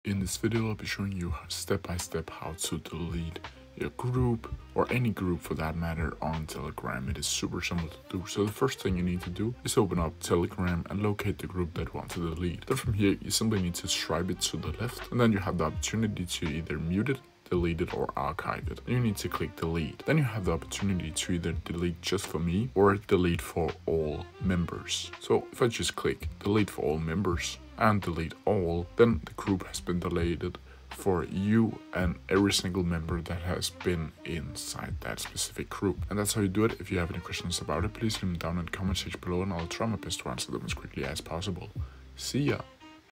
How To Delete Telegram Group (2025)
In this video I will shwo you step by step how to delete Telegram groups on Telegram App. With this tutorial you will learn how to delete your groups in two different ways. You can either delete them just for you, which means that the group will still be active, just without you in it or you can delete it for all members (if you are admin on the group). If you delete it for all members, every single member of your group will get kicked out and the group will be permanently deleted.